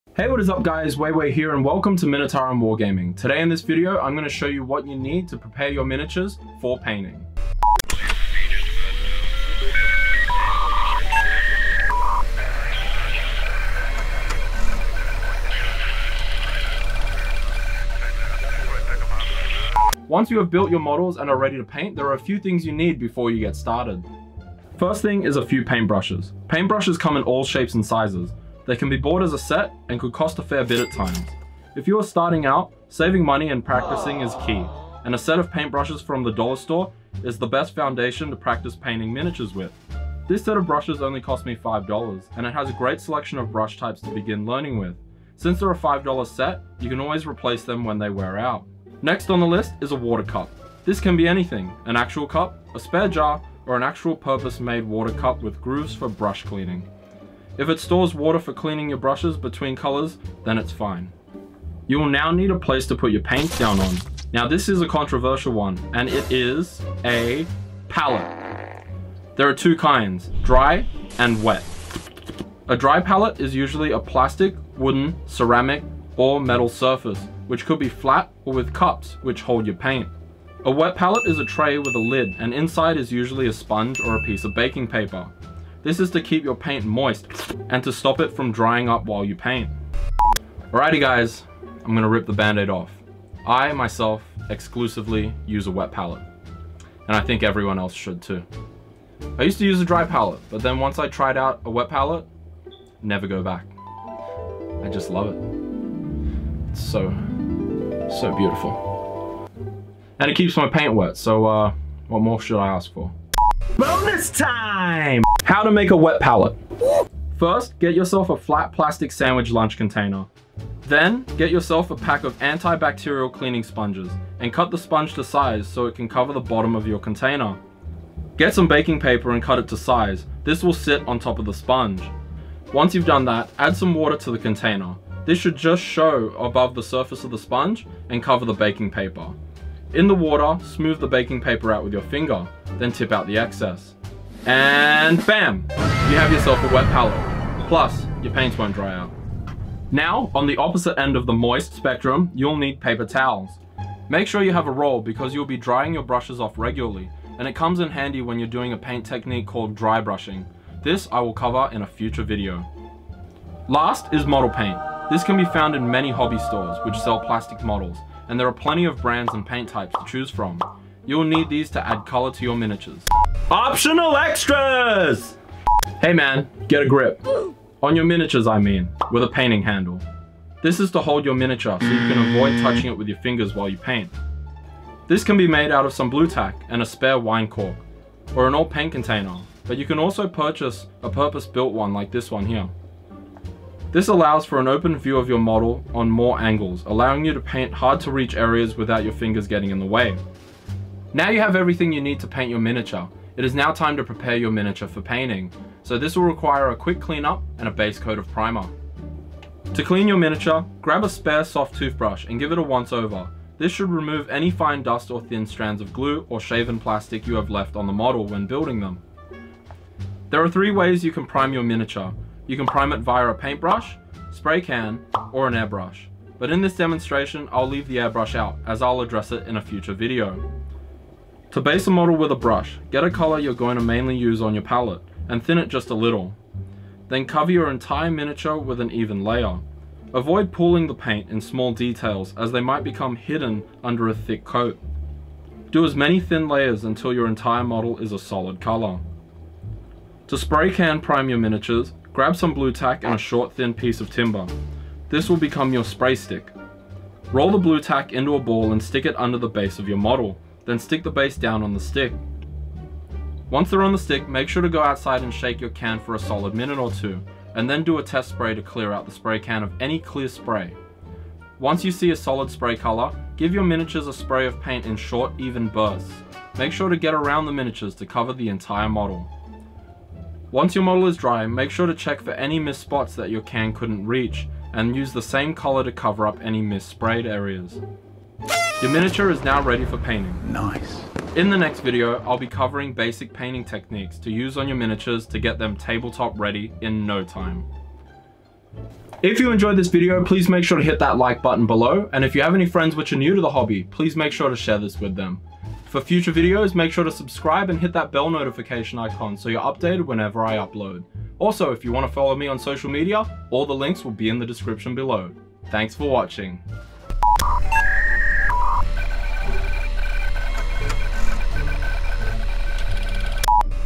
Minitarum Wargaming - Preparing Your Miniatures For Painting
Hey what is up! Wei Wei here and welcome to Minitarum Wargaming!

In this video, I show you what you will need to prepare your miniatures for painting.

In the next episode, I will be going over the basic painting techniques you will need to paint your models!

If you did not like the video, make sure to comment any improvements I can make for future videos.